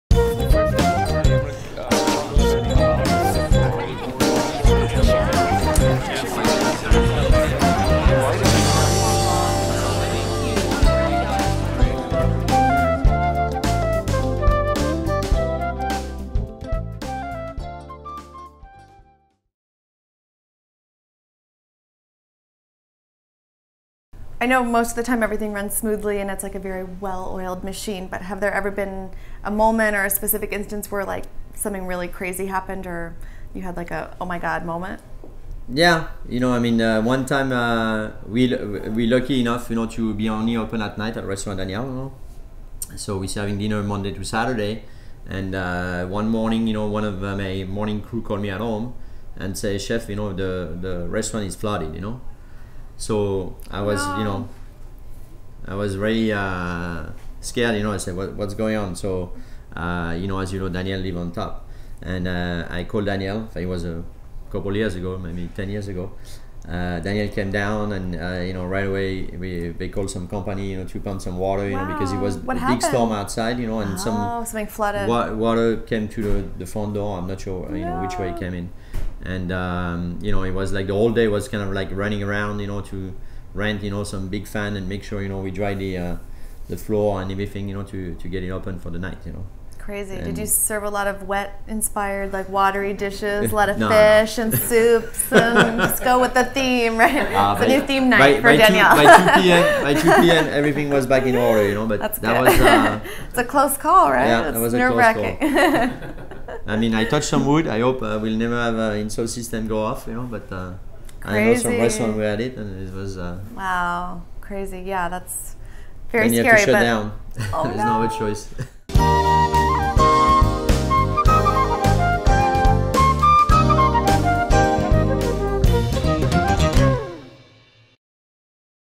Jean-Francois Bruel -O.M.G. Moment
Jean-Francois is the Exec Chef at Restaurant Daniel. The 100 year flood!